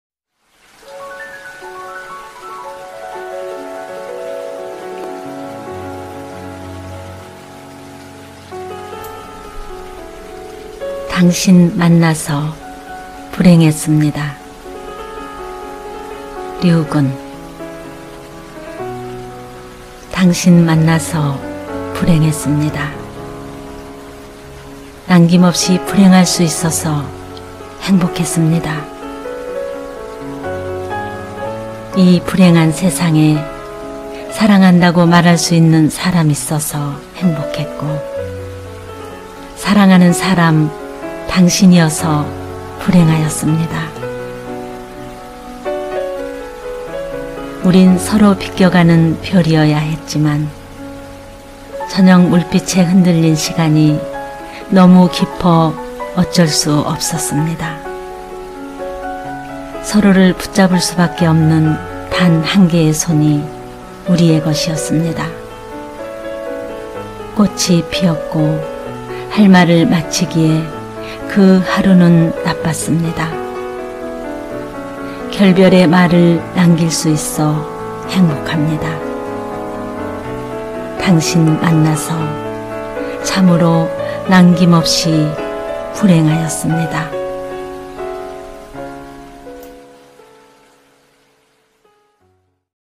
[시낭송]당신 만나서 불행했습니다/류근(낭송:봉경미)좋은시,사랑시,이별시,그리움의시,감성시,
‘시 한 편이면 상처도 꽃이 된다.’는 말이 있습니다.
그 꽃 위에 향기와 노래를 얹습니다.
바쁜 일상이지만 잠시 마음을 내려 놓으시고
시와 음악 그리고, 아름다운 영상과 함께
평안한 시간 보내시길 바랍니다.
귀한 걸음 해 주신 모든 님들 감사합니다.
늘 행복하시길요^^~

당신 만나서 불행했습니다

류근

남김 없이 불행 할수 있어서 행복 하였습니다.
이 불행한 세상에 사랑한다고 말할 수 있는 사람
있어서 행복했고
사랑하는 사람
당신이어서 불행하였습니다.
우린 서로 비껴가는 별이어야 했지만
저녁 물빛에 흔들린 시간이 너무 깊어 어쩔 수 없었습니다.
서로를 붙잡을 수밖에 없는 단 한 개의 손이 우리의 것이었습니다.
꽃이 피었고
할 말을 마치기에 그 하루는 나빴습니다.
결별의 말을 남길 수 있어 행복합니다.
당신 만나서 참으로 남김없이 불행하였습니다.

류근 시집
≪어떻게든 이별≫

 시인의 말

문학과 지성사 2016